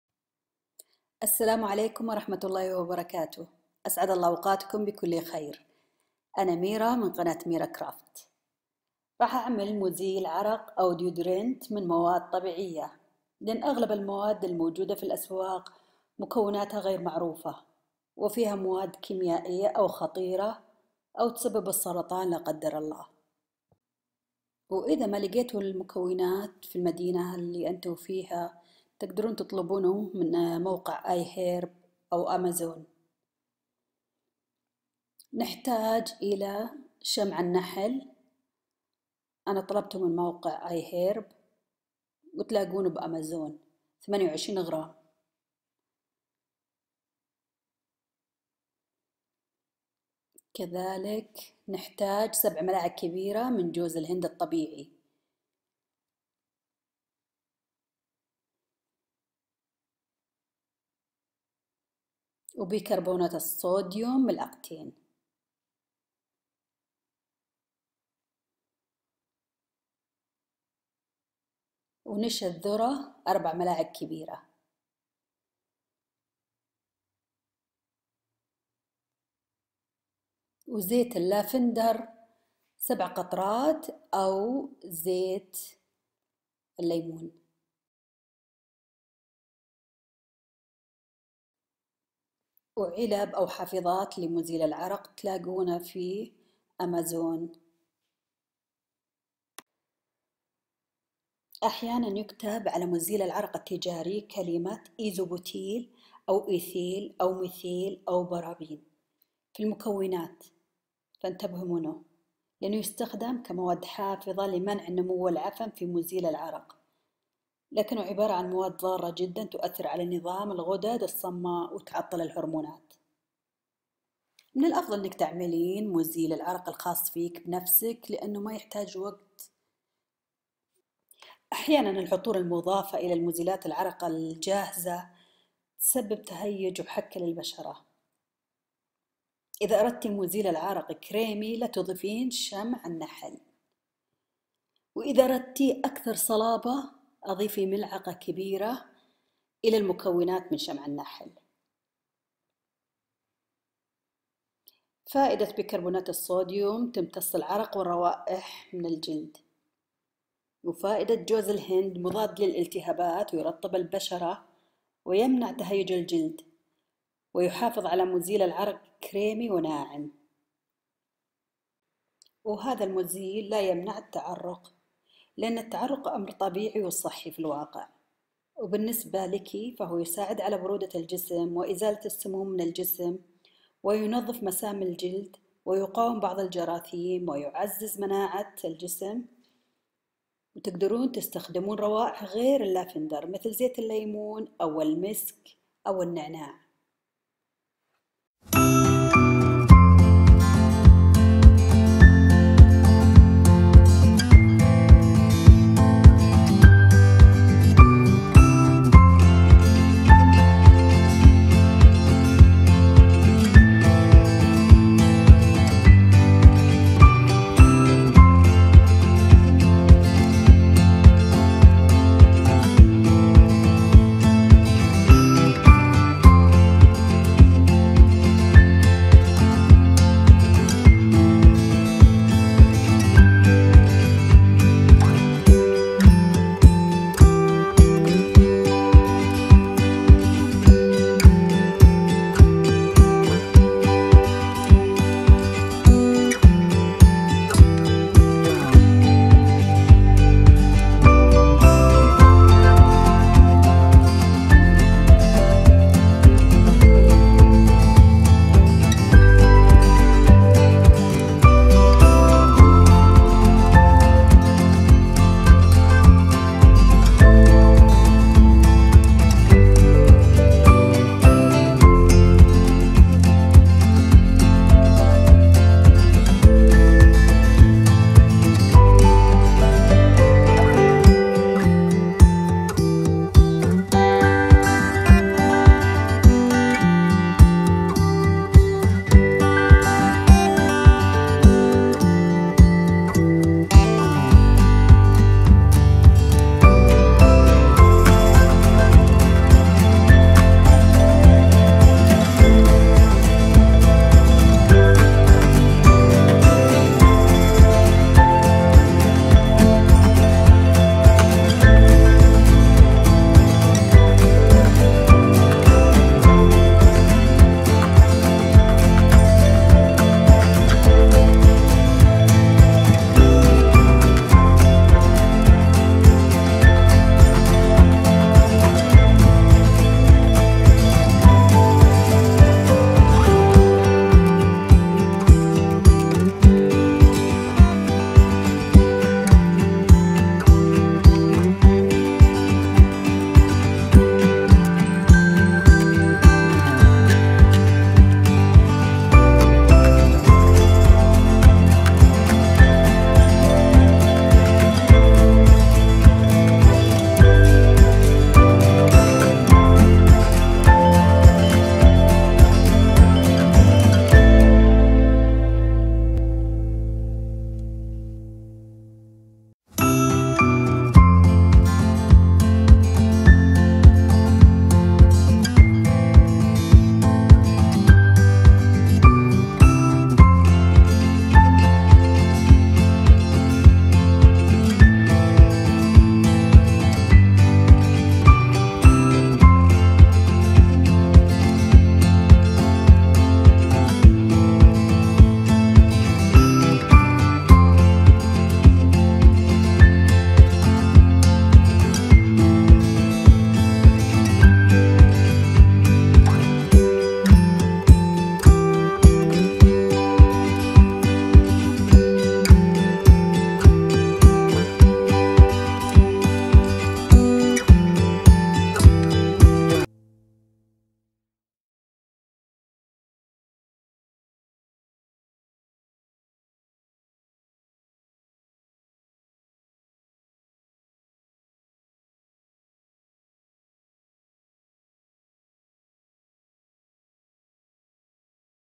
كيفية عمل مزيل عرق طبيعي How To Make Homemade Deodorant Bars
مكونات مزيل العرق
شمع نحل
زيت جوز الهند
بيكربونات الصوديوم
نشا الذرة
رائحة اللافندر
حافظات او علب لمزيل العرق

واتمنى مشاهدة باقي الفديوهات